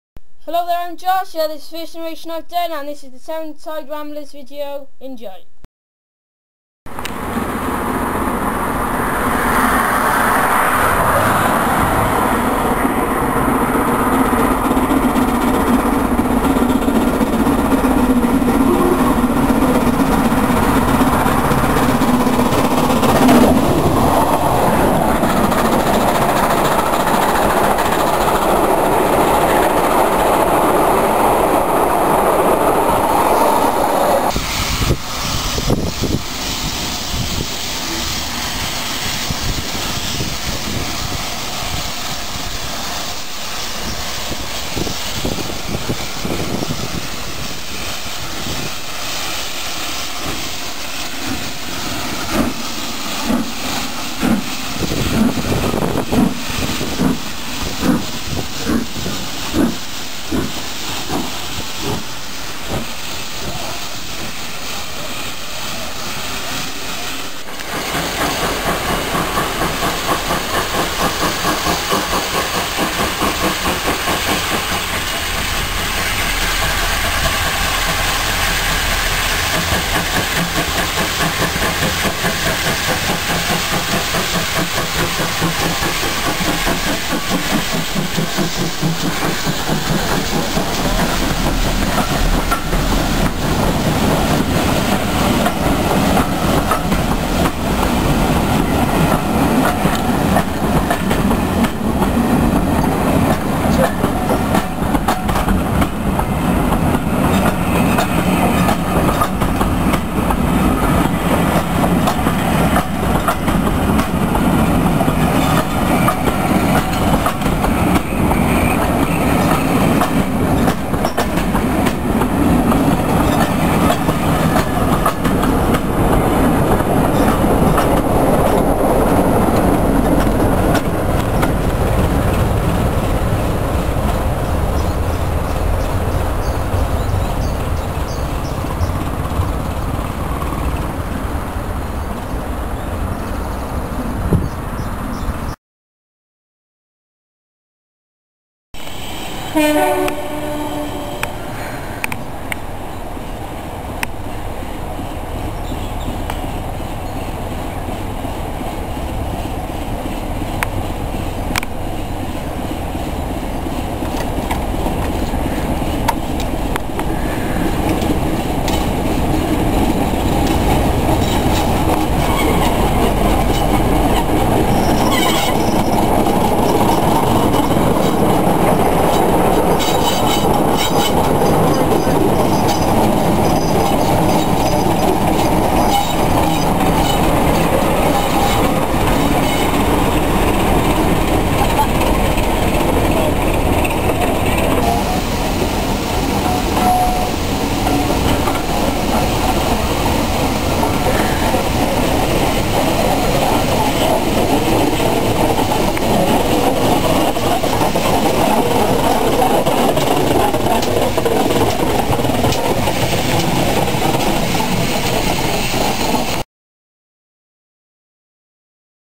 The Severnside Ramblers
Videos at different places of 'The Severnside Ramblers' during Sunday 22nd April 2007. First location is Sommerton Bridge about 1/4 mile from where I live. Second Location - Lydney Junction, station on the Dean Forest Railway and the last location is Norchard High Level station.